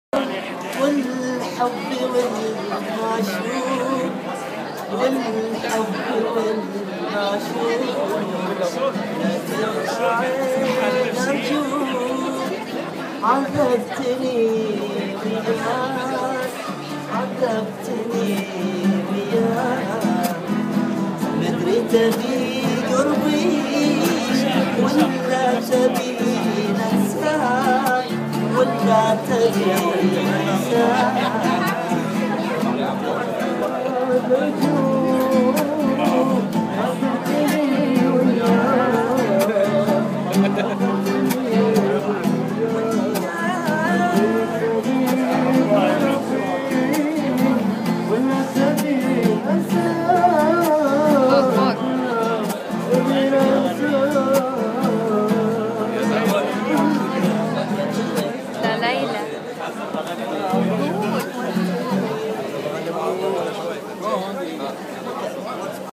عرض غناء وعزف في جامعة البحرين لطلبة الجامعة
تصوير بحرين يونايتد

العرض مقدم من طلبة جامعة البحرين